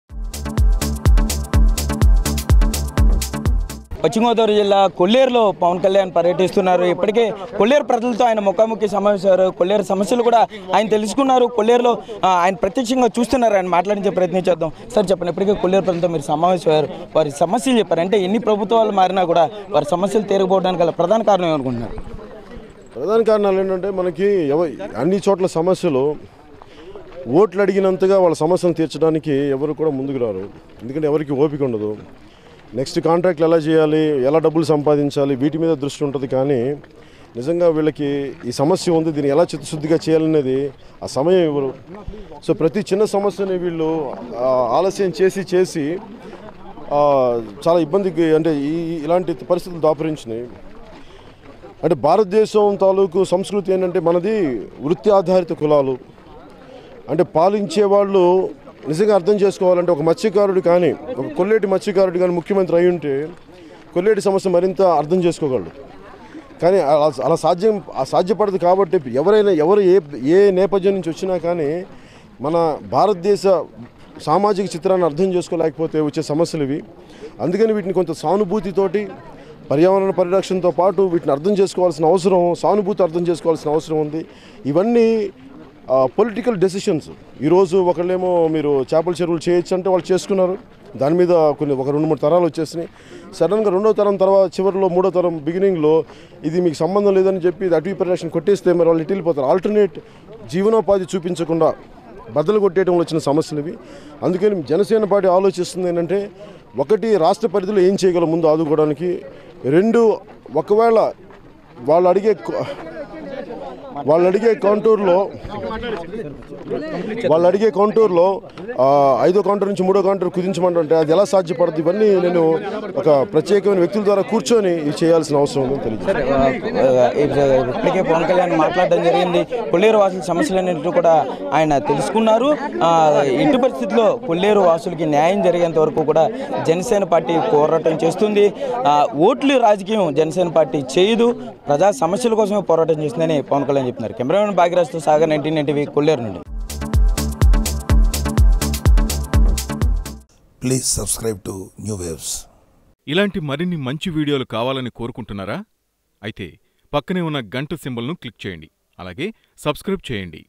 కొల్లేరు సమస్యలపై పవన్ Face to Face with Pawan Kalyan over Kolleru Problems | New Waves
face to face with janasena chief pawan kalyan over kolleru problems